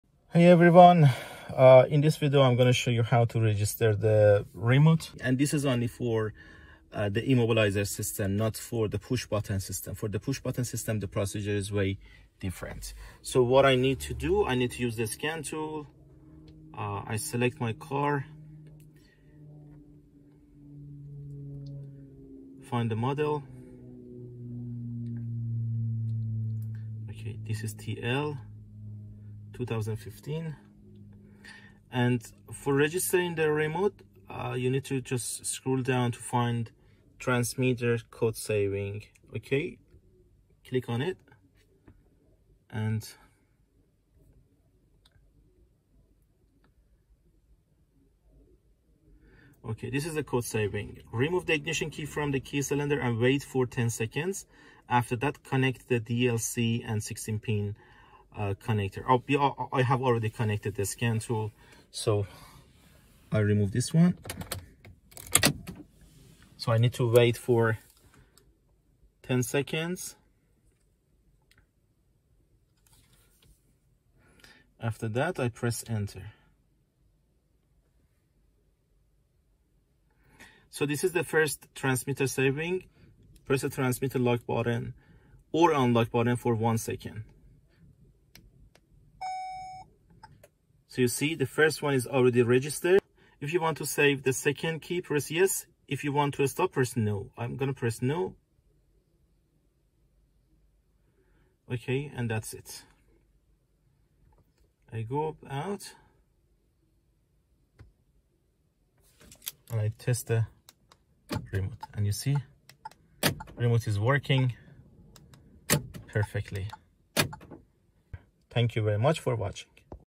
How to Re-Program Remote on Hyundai & KIA Vehicles | Without Any PIN CODE !
In this video I show you how I normally Re-Program Remote on Hyundai & KIA Vehicles